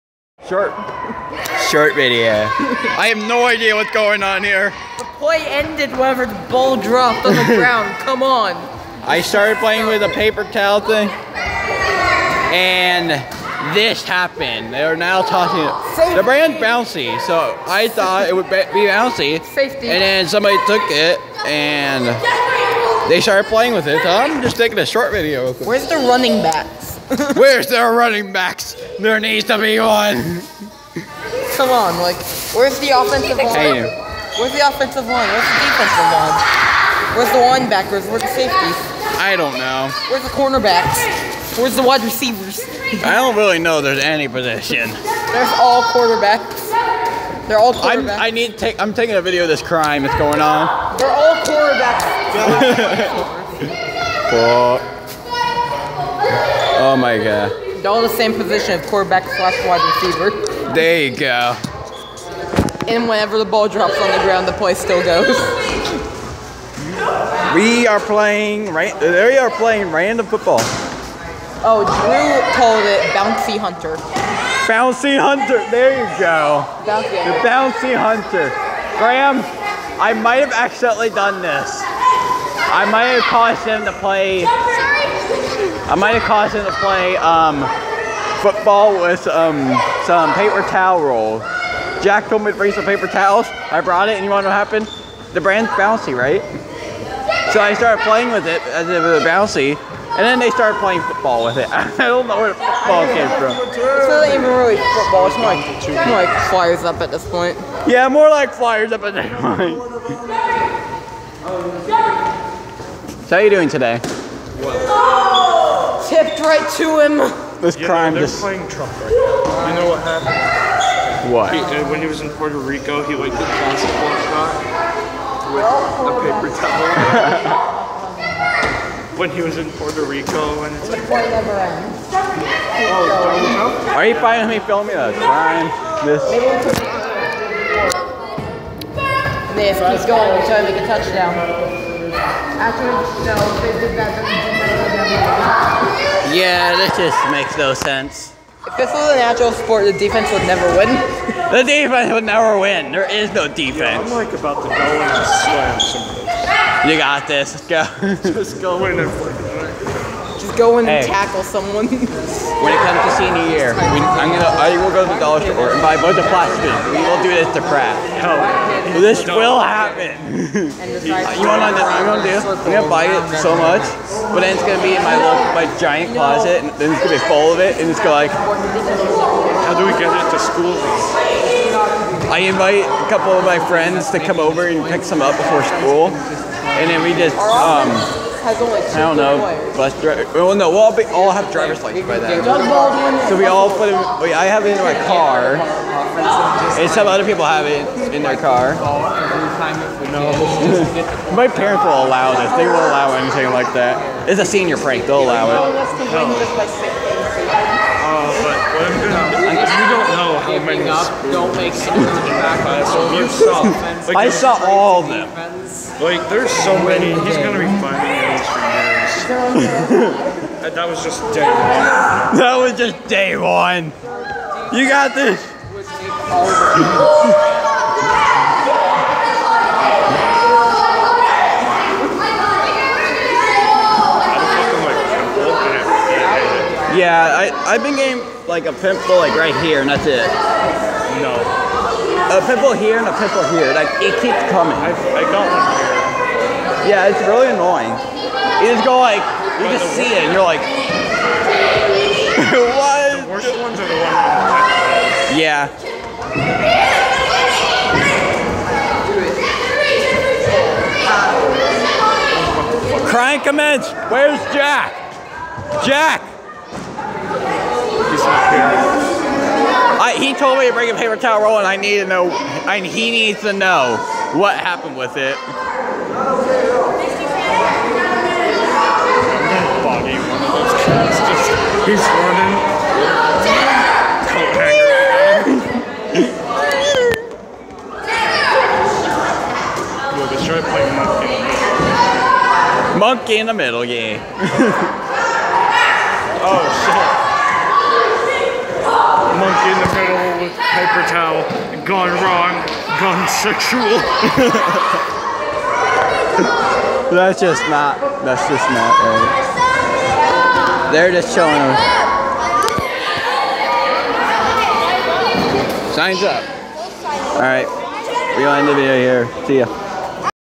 Football with paper towels!?!
A group of the newest boy scout in pack 577 started to play football with the paper towel roll!